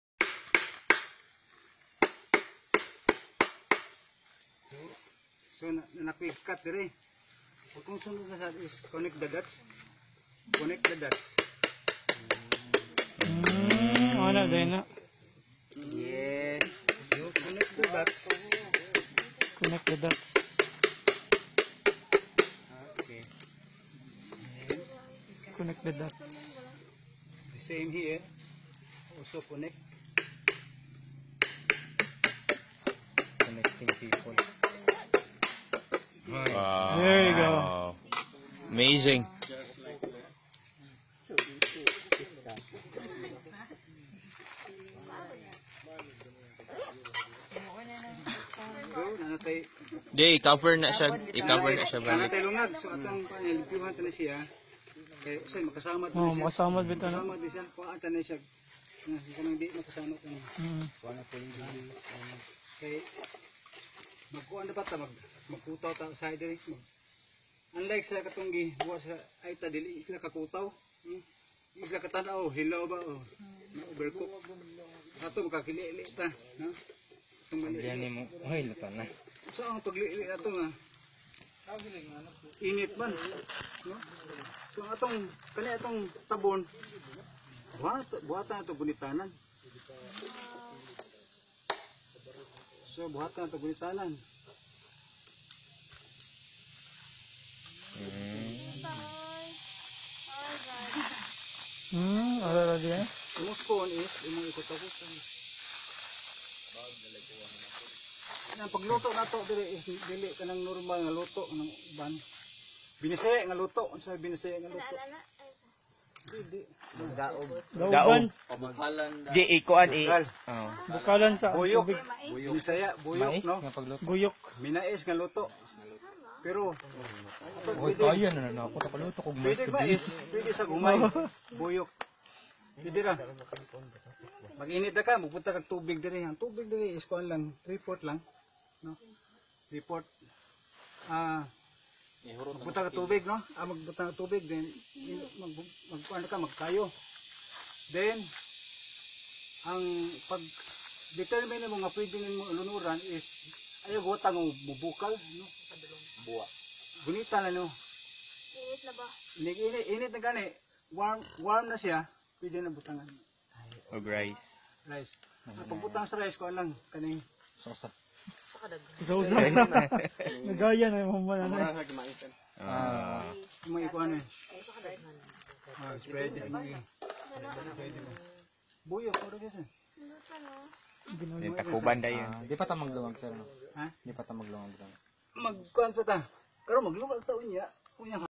The Trailhawk System
Making a bamboo cooking vessel applying the Trailhawk system.

Video courtesy of Denzen Delima.